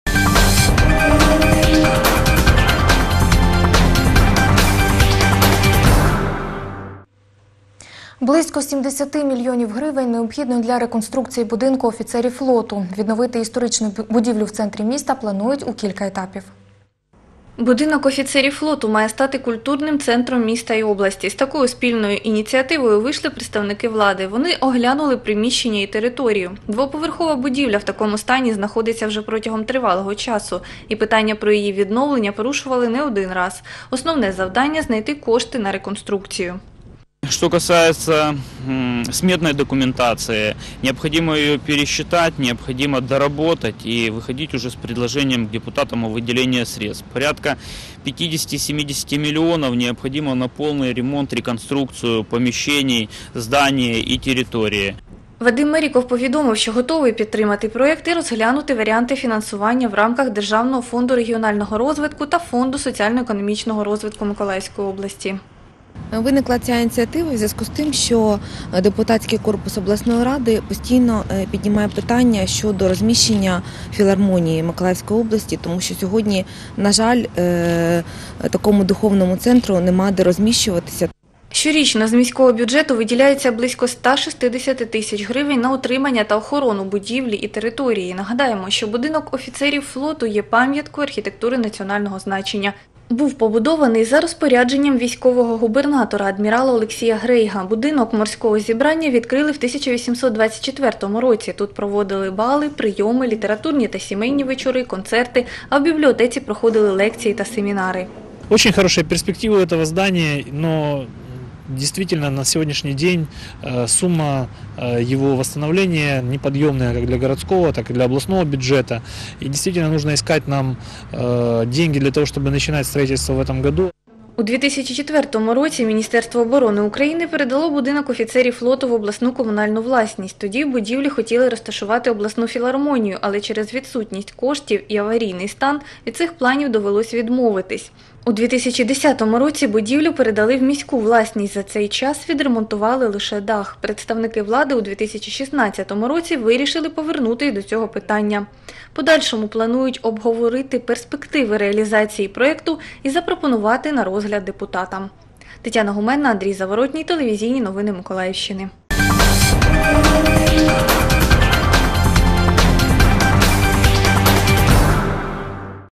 ТНМ сюжет - Будинок офіцерів флоту 24.02.2016
Близько 70 мільйонів гривень необхідно для реконструкції Будинку офіцерів флоту в Миколаєві.

Автор: Тетяна Гуменна.